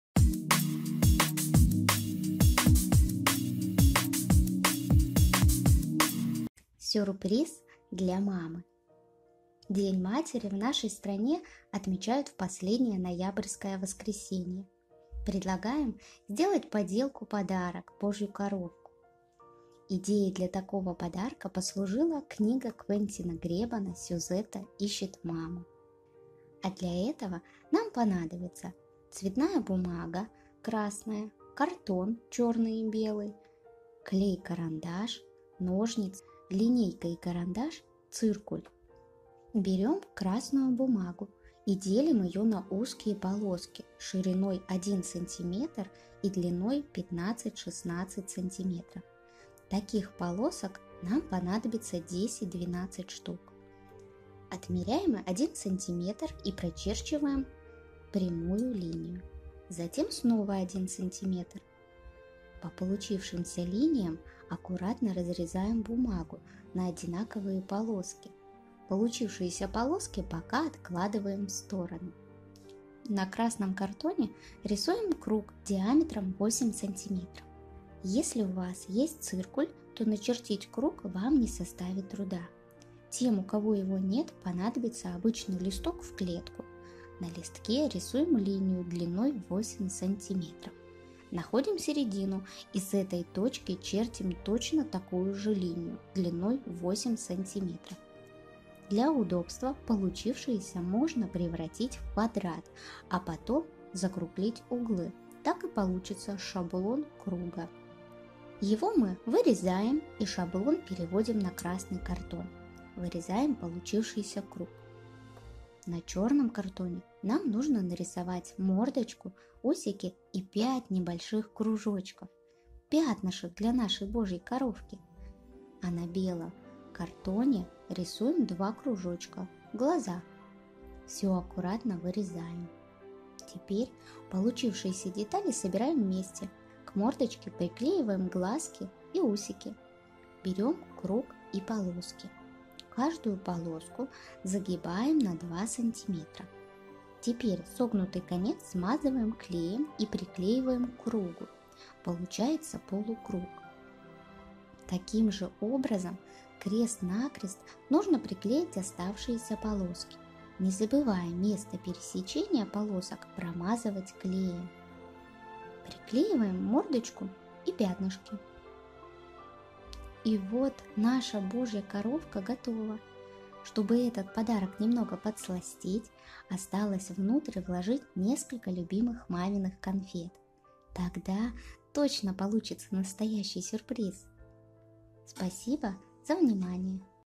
Поделка «Сюрприз для мамы»
В День матери Библиотека №15 (ул. Калевалы, д. 6) пригашает на мастер-класс по изготовлению сюрприза для мамы – поделки в виде бумажной божьей коровки.